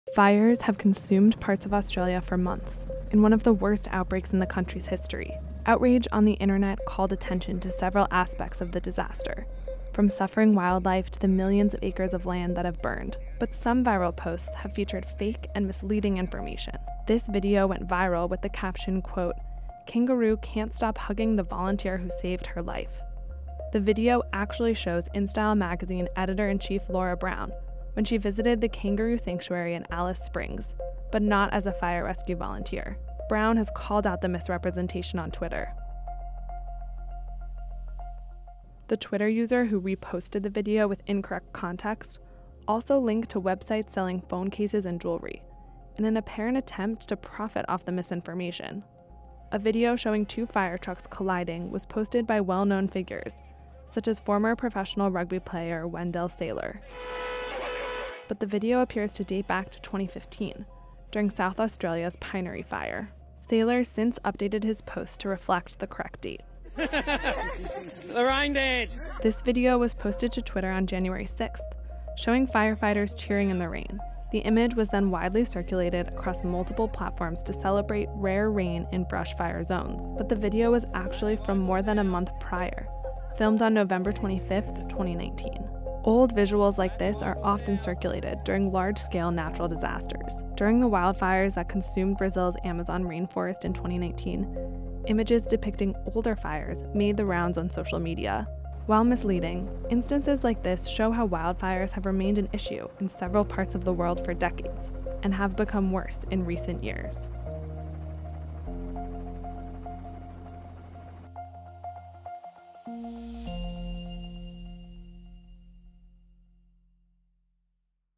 Fake and misleading images of the Australia bush fires, debunked
Fires have consumed parts of Australia for months in one of the worst outbreaks in the country’s history.

Outrage on the Internet called attention to several aspects of the disaster — from suffering wildlife to the millions of acres of land that have been burned. But some viral posts have featured fake and misleading information.

Misleading visuals are often circulated during large- scale natural disasters. During the wildfires that consumed Brazil’s Amazon Rainforest in 2019, images depicting older fires made the rounds on social media.

While some of these posts did not provide proper context, instances like this show how wildfires have remained an issue in several parts of the world for decades — and have become worse in recent years.